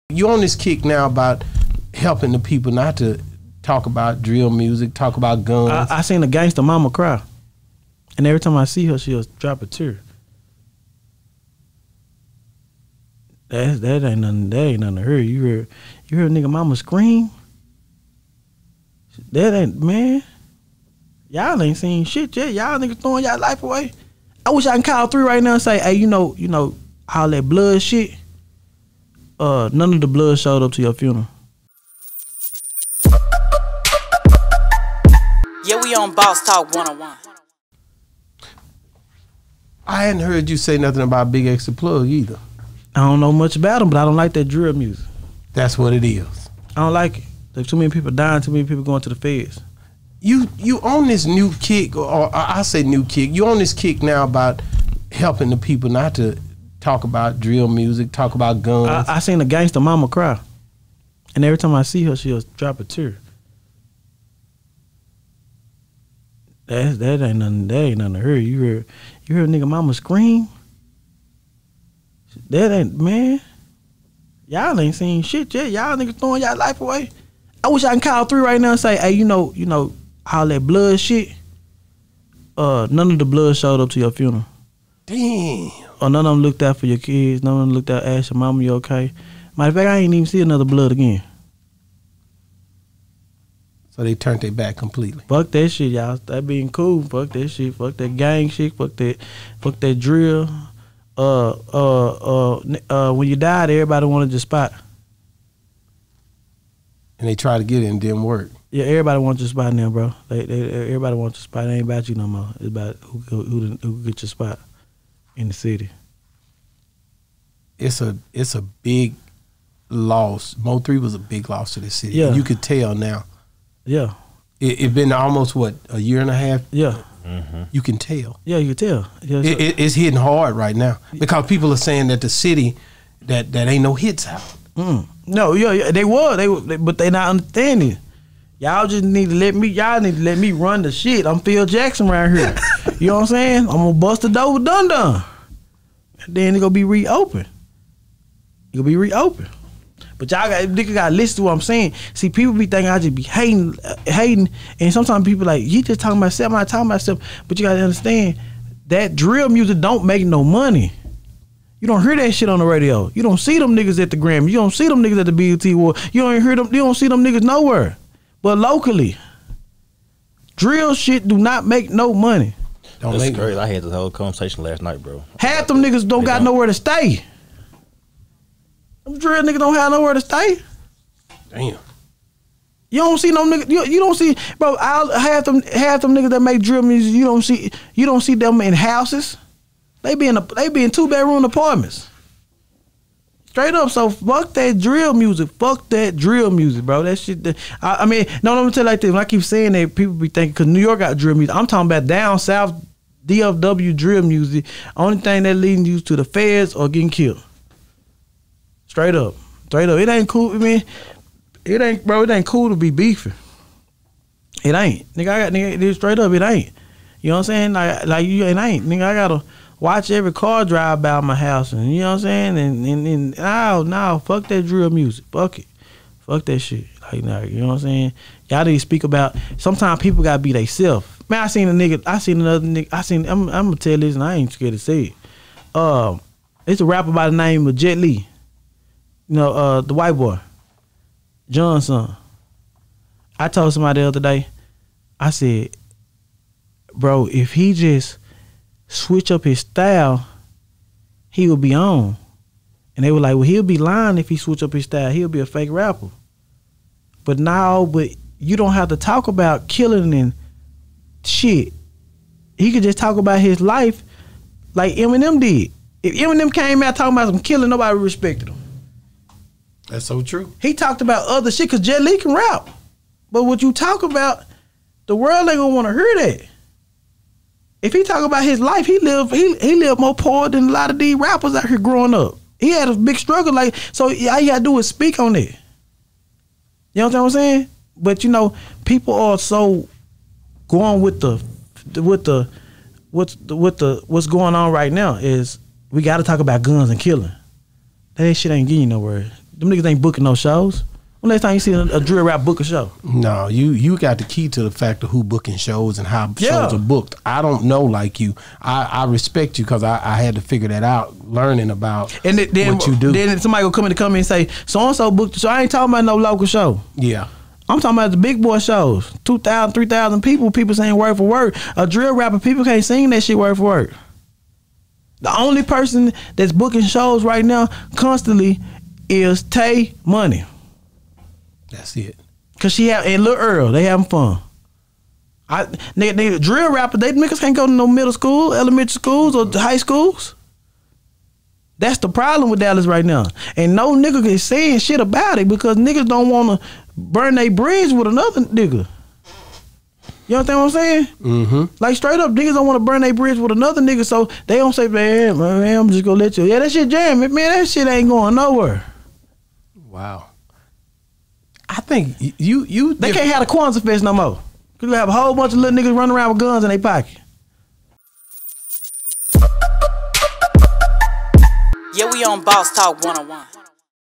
Rainwater on Mo3 "I Seen a Gangsta Momma Cry" and Every Time I see her she Drop a Tear (Part 12)
Rainwater on Mo3 his Mom and what comes with Gangsta music and the Drill Music